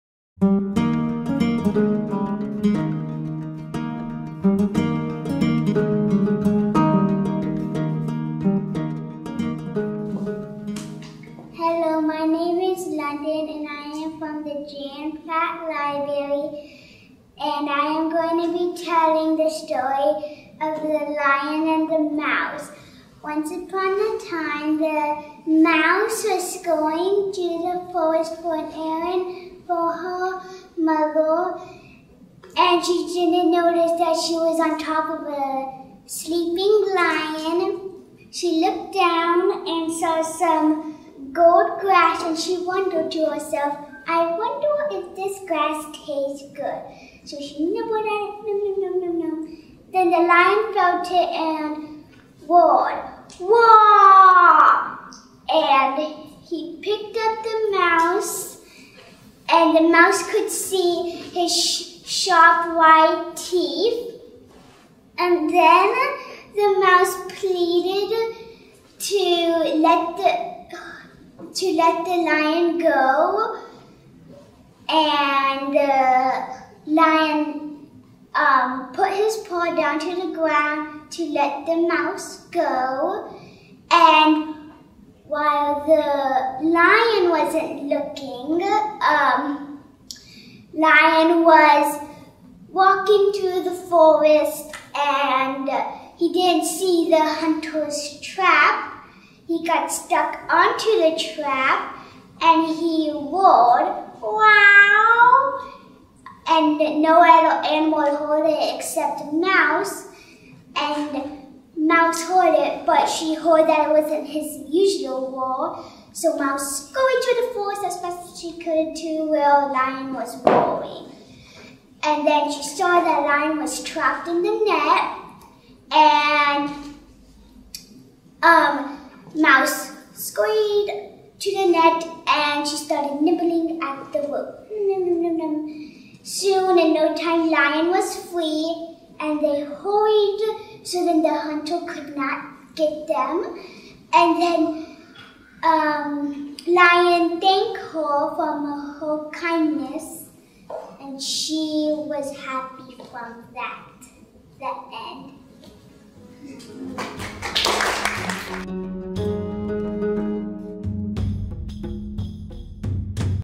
44th Annual Tampa-Hillsborough County Storytelling Festival - Londyn
Enjoy this Festival Quality performance from the 44th Annual Tampa-Hillsborough County Storytelling Festival by young storyteller, Londyn.